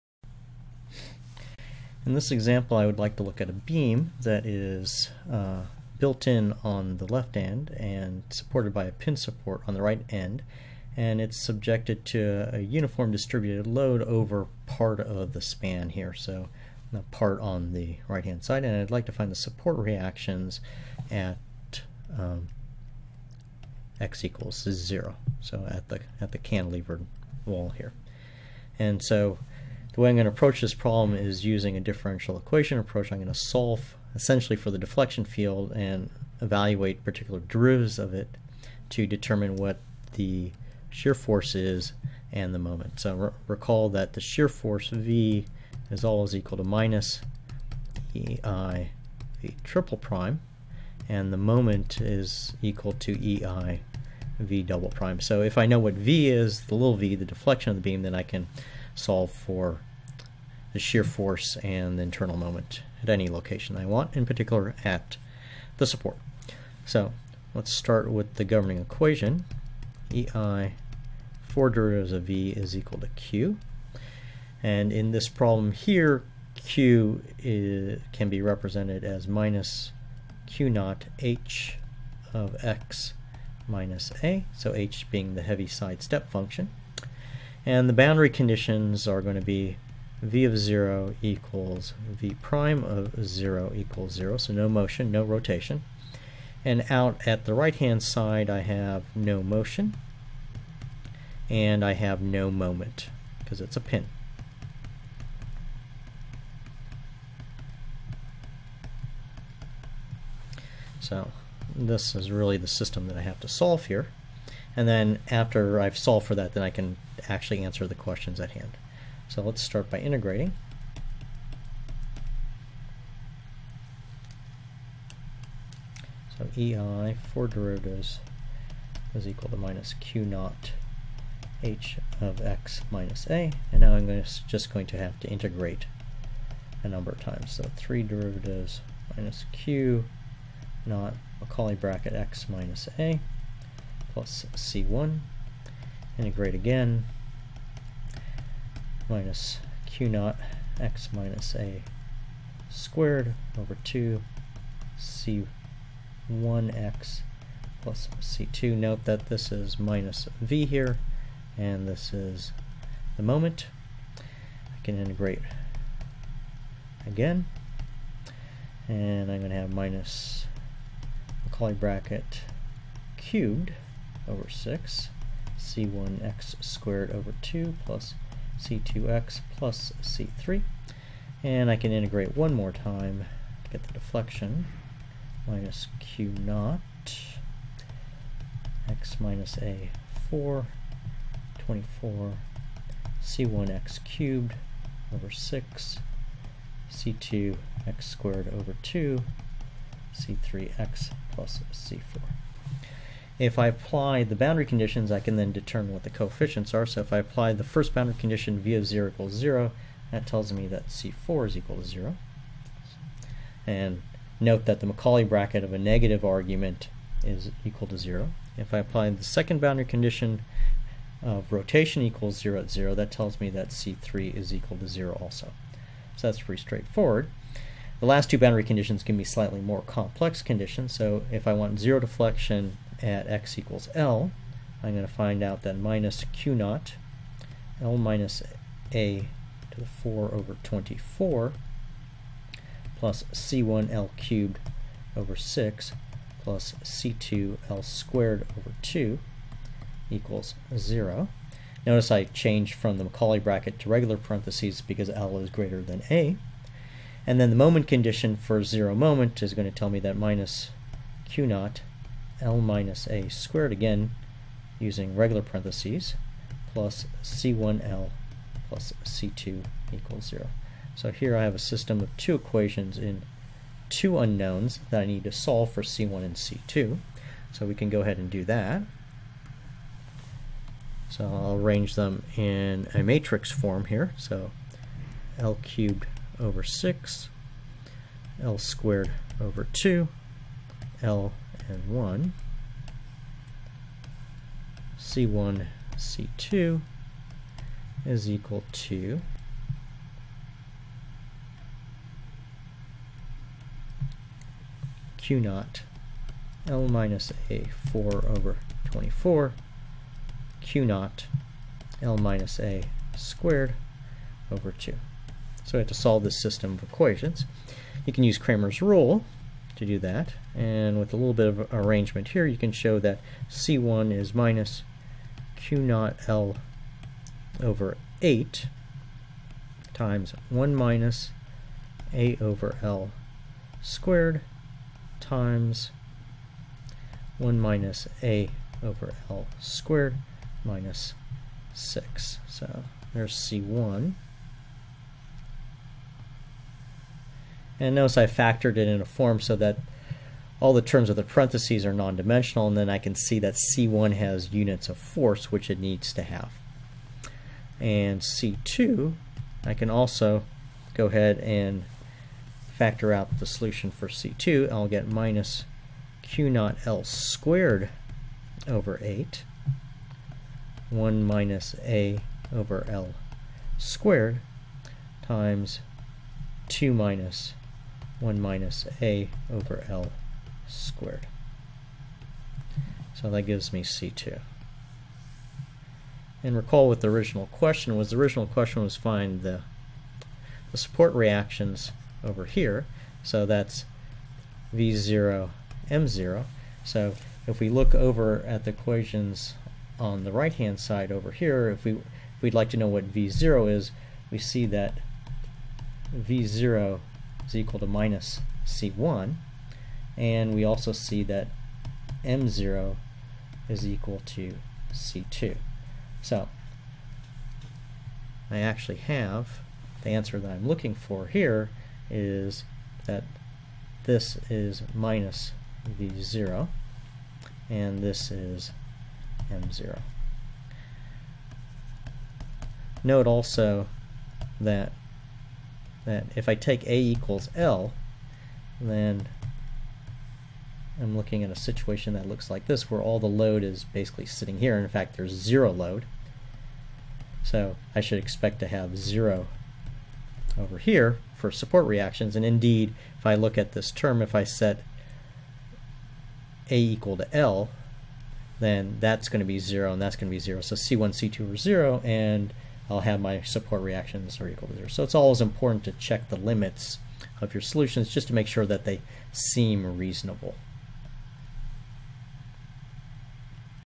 Beam bending example, statically indeterminate
Example determination of the deflection of a statically indeterminate beam using a differential equation method, including the determinate of the support reactions